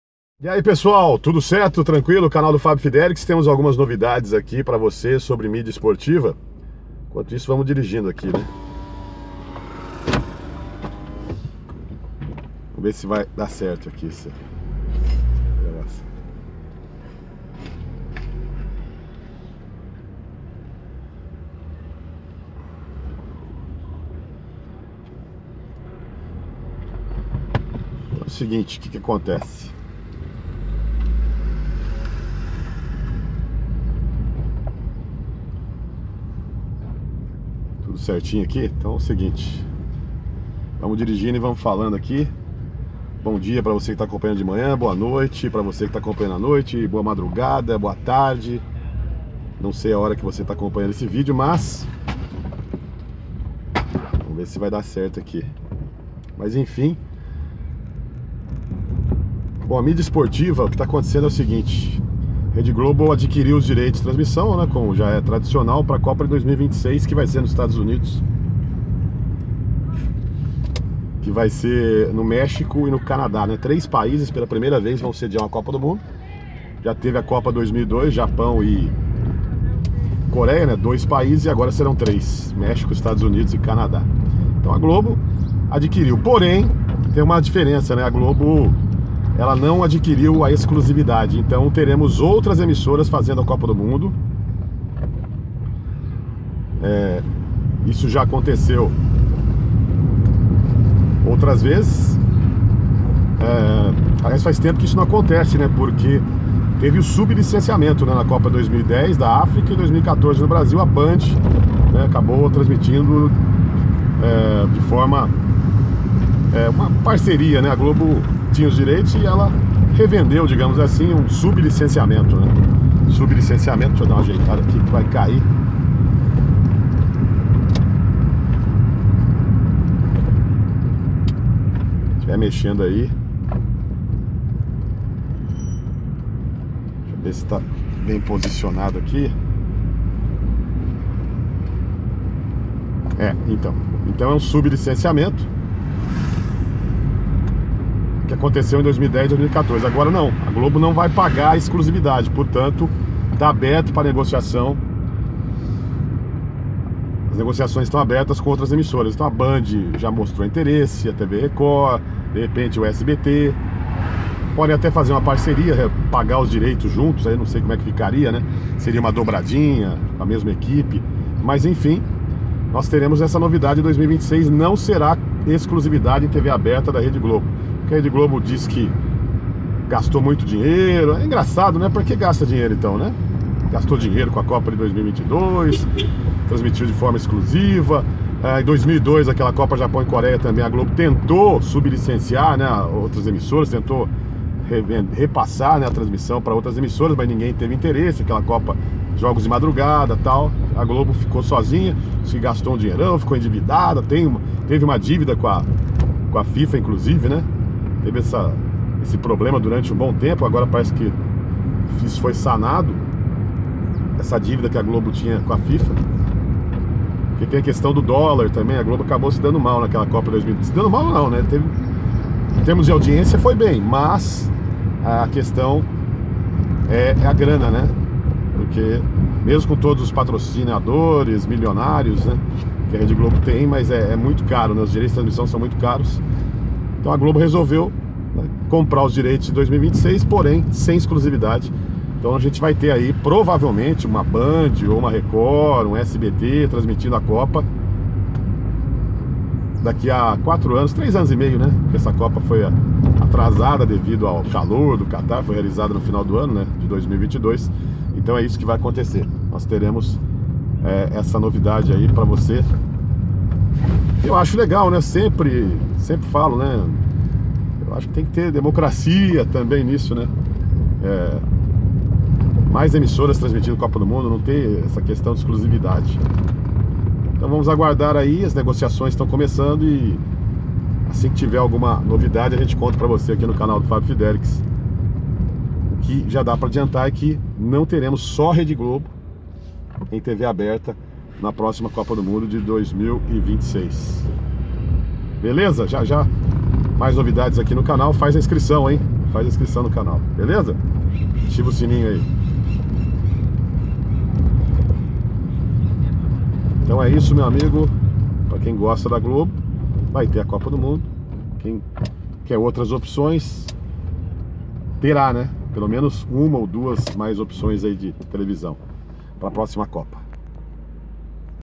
GLOBO NÃO TERÁ EXCLUSIVIDADE EM 2026!
NA COPA DE 2026 A REDE GLOBO NÃO TERÁ EXCLUSIVIDADE EM TV ABERTA.